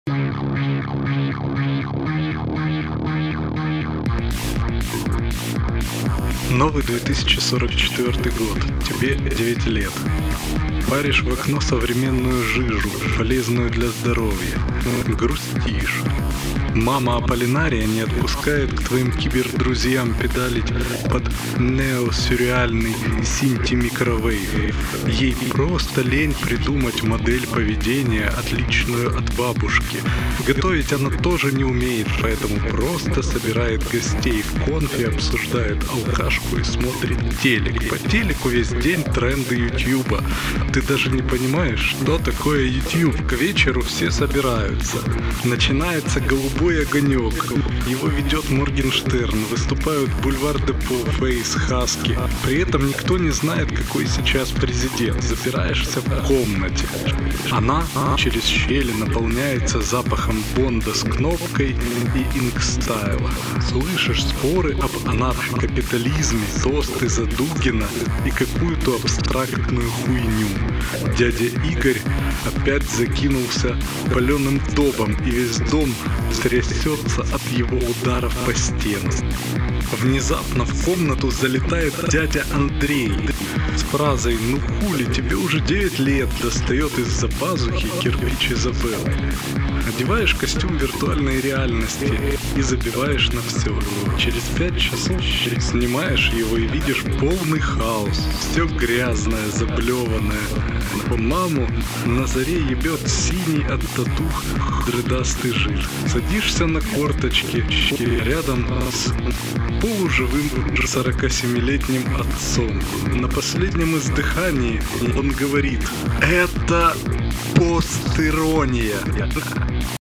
Новый 2044 год (киберпанк который мы заслужили)
Новый 2044 год
Бессмысленный и беспощадный русский киберпанк.
Читает и шумит NoiseBook
Просто текст понравился, с прошлого года его хранил.
В оформлении использована работа Евгения Зубкова.